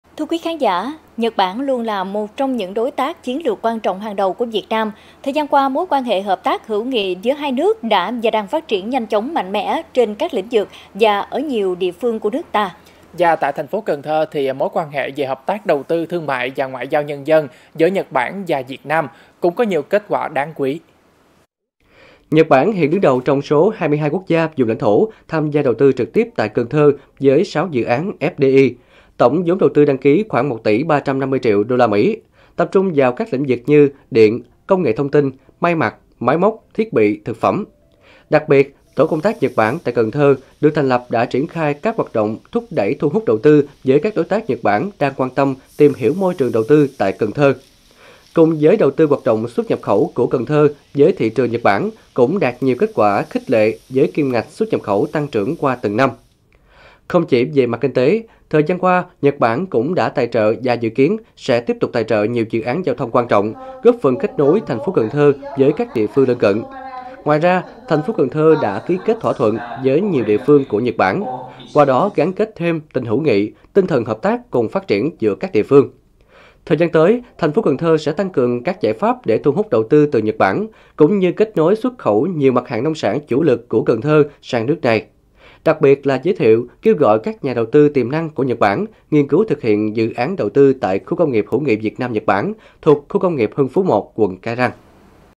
Kết quả hợp tác Nhật Bản - Việt Nam trên đất Tây Đô | Cần Thơ TV
Nhật Bản luôn là một trong những đối tác chiến lược quan trọng hàng đầu của Việt Nam. Thời gian qua, mối quan hệ hợp tác, hữu nghị giữa hai nước đã và đang phát triển nhanh chóng, mạnh mẽ trên các lĩnh vực và ở nhiều địa phương của nước ta. Tại TP. Cần Thơ, mối quan hệ về hợp tác đầu tư, thương mại và ngoại giao nhân dân giữa Nhật Bản và Việt Nam cũng có nhiều kết quả hết sức đáng quý.